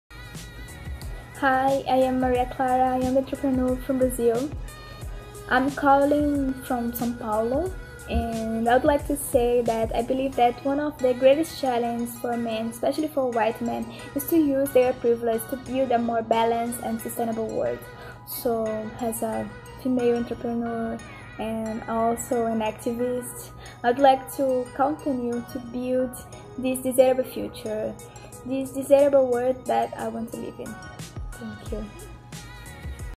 Maria Clara Magalhres | PV Exclusive Collectors Edition, Men Leaders to Look Upto - 2020
Checkout the latest Magazine of  Passion Vista’s Exclusive Collectors Edition of Men Leaders to Look Upto 2020 celebrating International Mens day with magnificent men from across the globe.